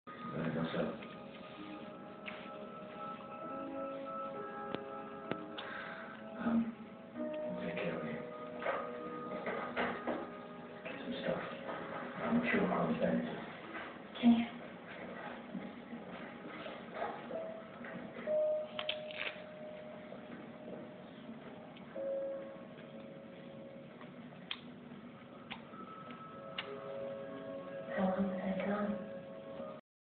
Demo plays dead
My lazy cat refuses to move.